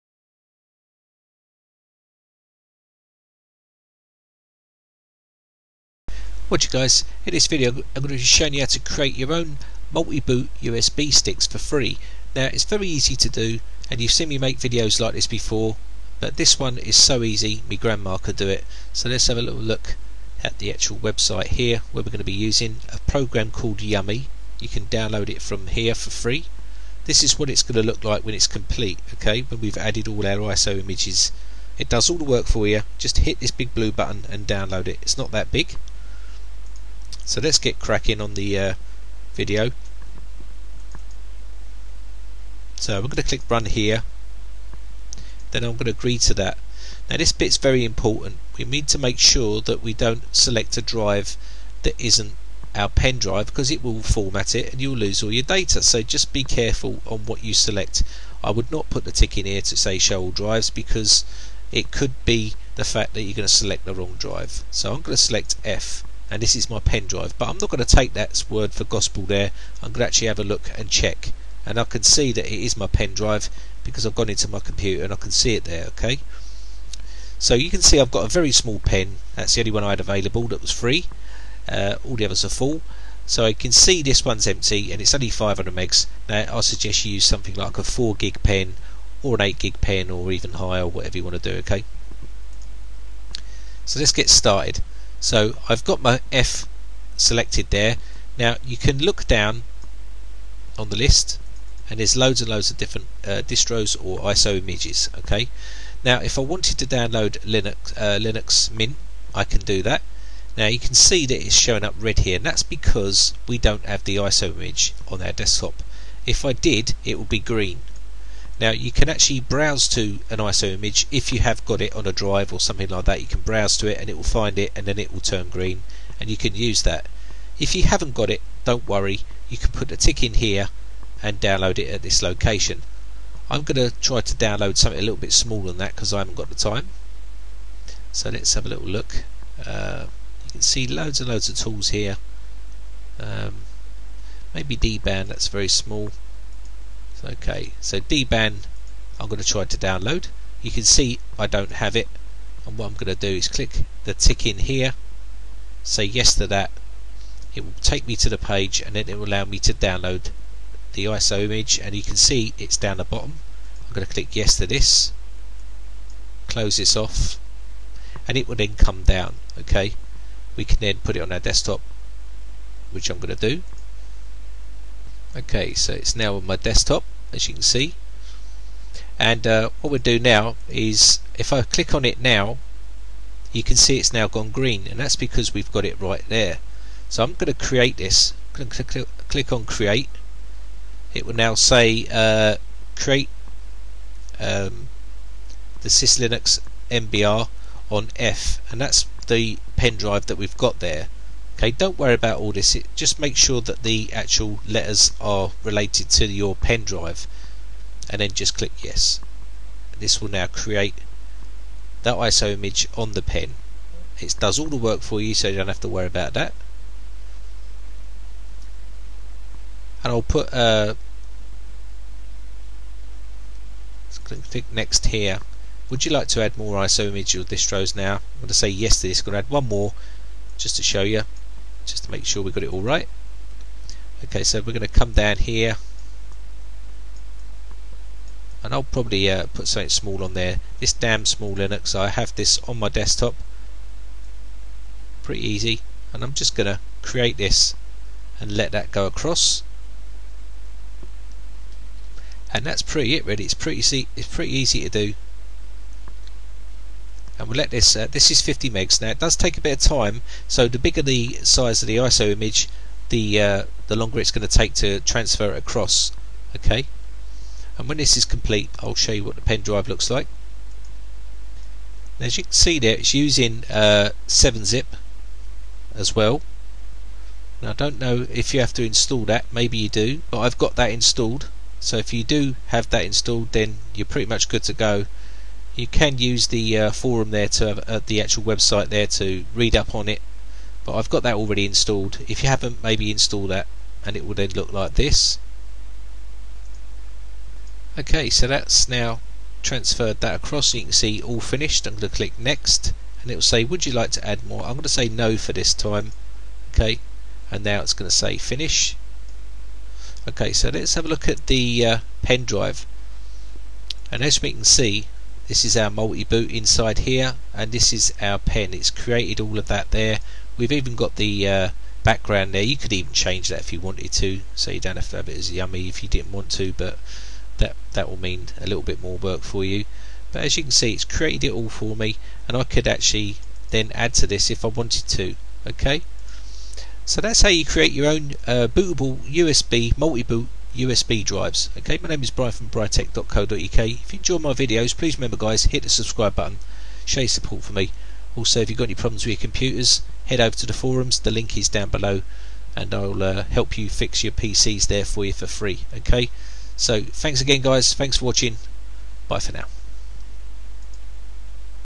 Build A Multiboot USB Drive For Free with Yumi by Britec
Build A Multiboot USB Drive For Free with Yumi

YUMI (Your Universal Multiboot Installer), is the successor to MultibootISOs. It can be used to create a Multiboot USB Flash Drive containing multiple operating systems, antivirus utilities, disc cloning, diagnostic tools, and more. Contrary to MultiBootISO's which used grub to boot ISO files directly from USB, YUMI uses syslinux to boot extracted distributions stored on the USB device, and reverts to using grub to Boot Multiple ISO files from USB, if necessary.

Aside from a few distributions, all files are stored within the multiboot folder, making for a nicely organized Multiboot Drive that can still be used for other storage purposes.

Creating a YUMI Multiboot MultiSystem USB Flash Drive
YUMI works much like our Universal USB Installer, except it can be used to install more than one distribution to run from your USB. Distributions can also be uninstalled using the same tool!